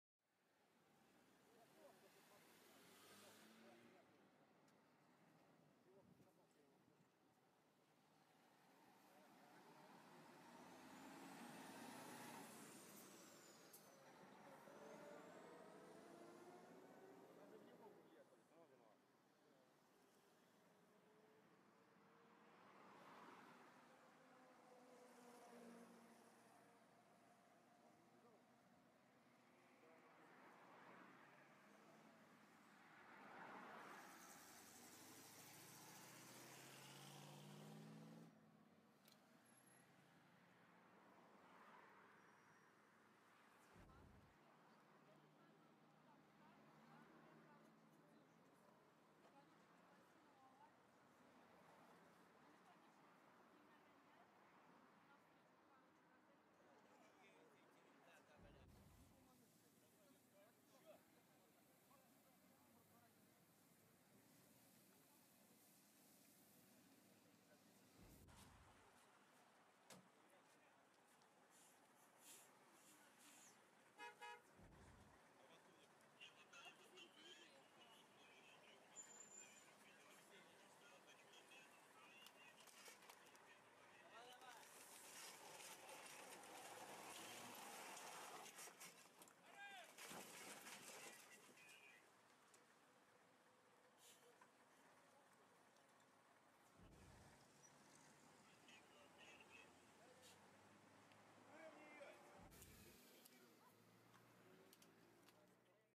Газель врезалась в Фольксваген на Горьковском шоссе в Казани
ДТП 9 июня 2014 года. На выезде из Казани Газель врезалась в Фольксваген